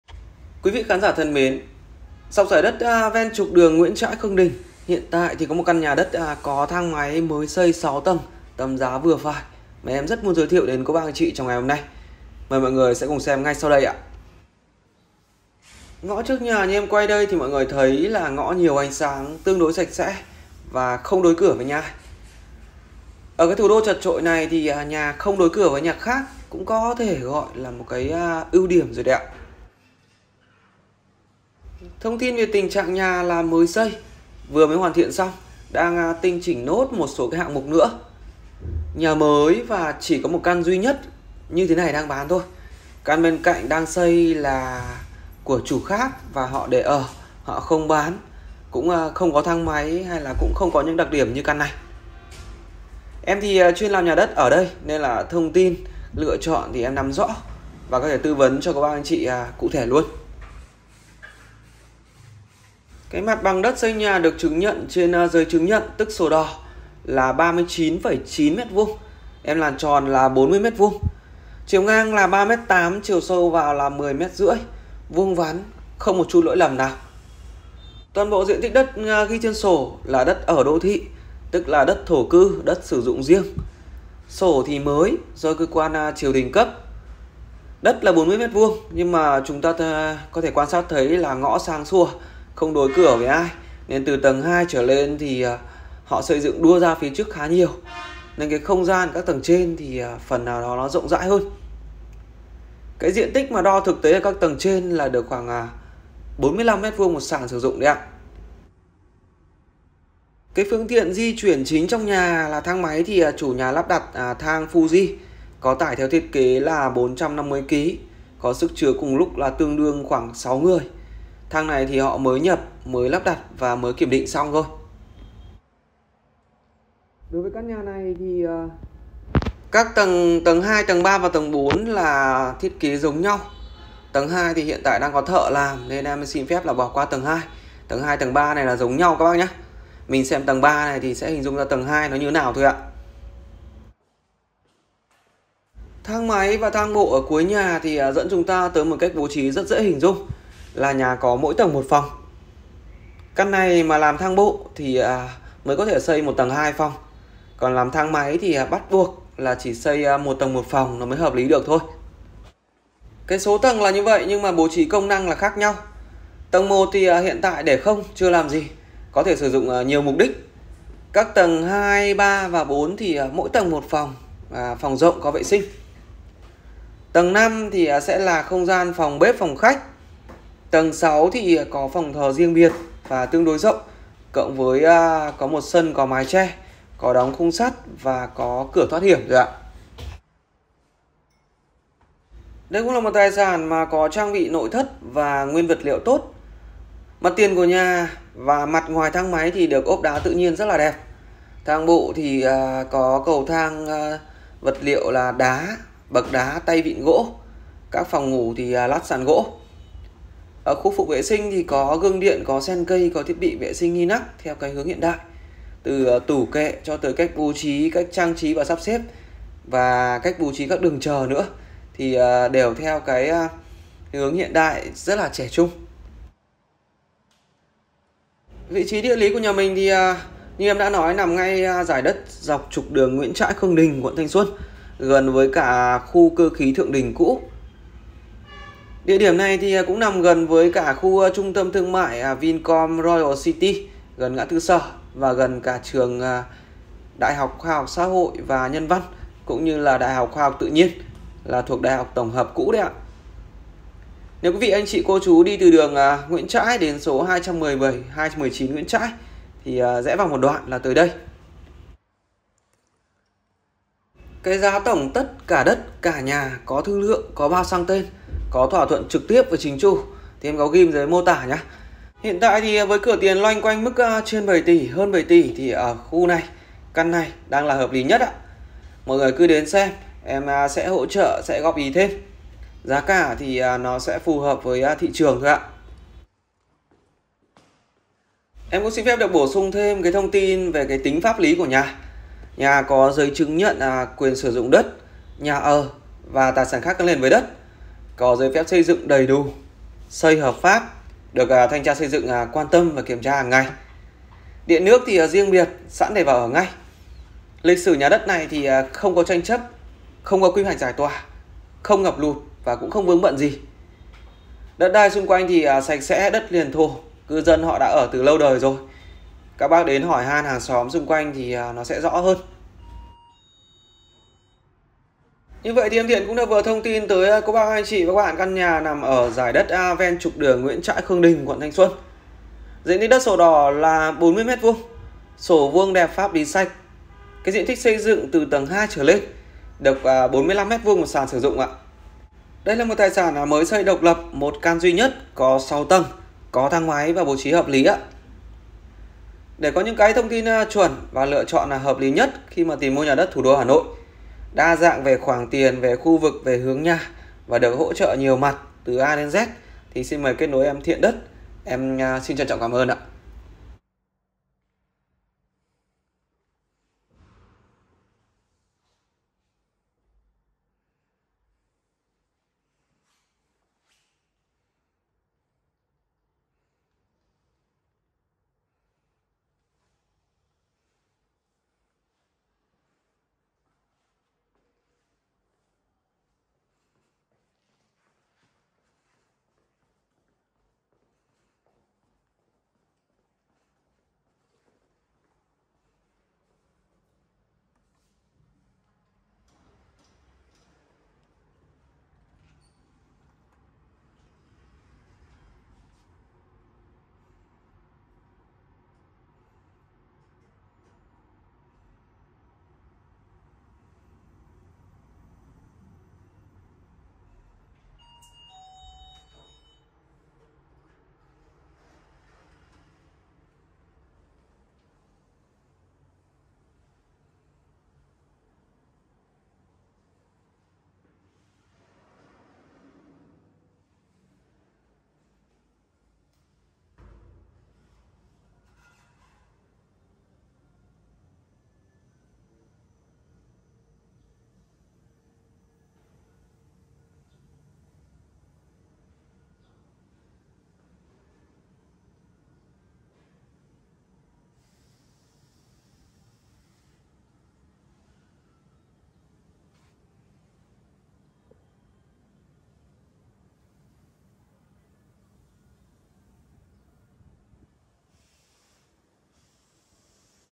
NHÀ ĐẤT 217 NGUYỄN TRÃI - KHƯƠNG ĐÌNH 40M2 6 TẦNG THANG MÁY
Bán nhà đất sổ đỏ chính chủ tại địa chỉ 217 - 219 Nguyễn Trãi - phố Khương Đình, Thanh Xuân, Hà Nội.

- Diện tích: 40m2 sổ đỏ; xây dựng tầng 2 trở lên: 45m2/ sàn
- Nhà mới độc lập, 1 căn duy nhất
- Giá bán: 7,x tỷ ( thương lượng )
- Thang máy Fuji 450kg - 6 người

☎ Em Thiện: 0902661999
Trân trọng!